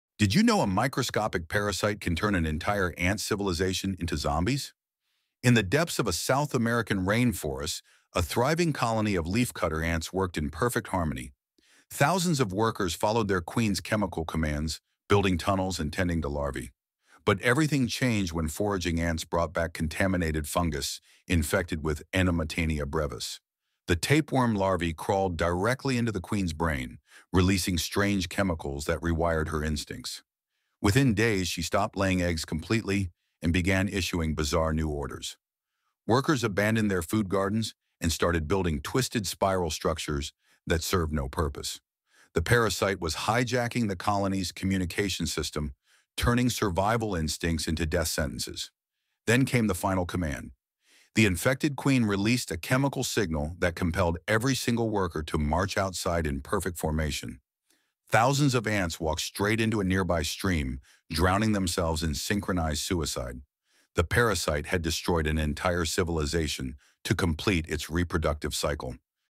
This Parasite Turns Entire Ant Colonies Into Mind-Controlled Zombies
parasite controls ant queen's mind = zombie colony 🐜🧠